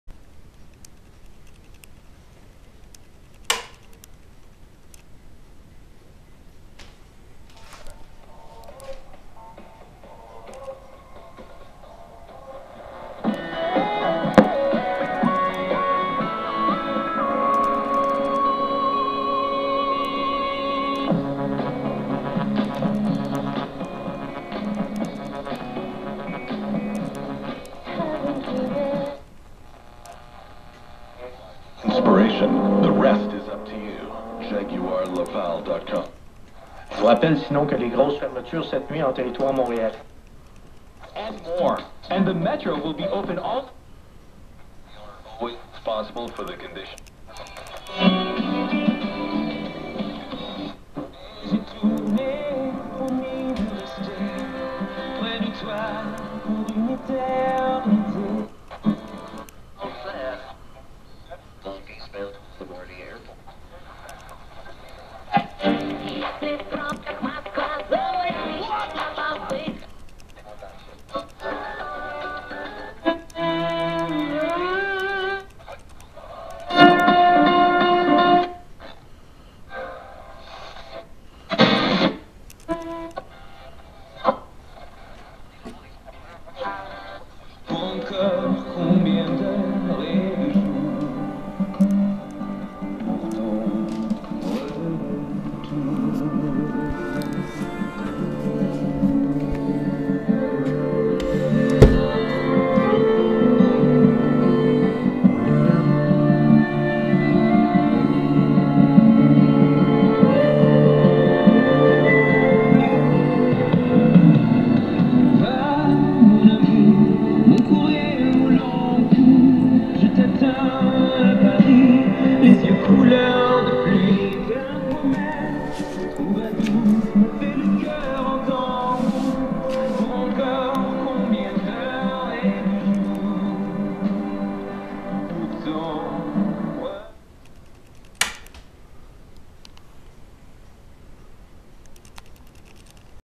Radio CORONADO model 740 de 1937
Country:  United States of America (USA)
Manufacturer / Brand:  Gamble-Skogmo, Inc.; Minneapolis, MN
Brand

Coronado
Year: 1937 ?? Category: Broadcast Receiver - or past WW2 Tuner
Valves / Tubes 6: 6A8G 6K7 6Q7G 6C5 6F6G 5Y3G
Main principle Super-Heterodyne (Super in general); ZF/IF 465 kHz
Wave bands Broadcast, Short Wave(s) and Police.
Details
Power type and voltage Alternating Current supply (AC) / 115 Volt
Loudspeaker Electro Magnetic Dynamic LS (moving-coil with field excitation coil) / Ø 6 inch = 15.2 cm
Power out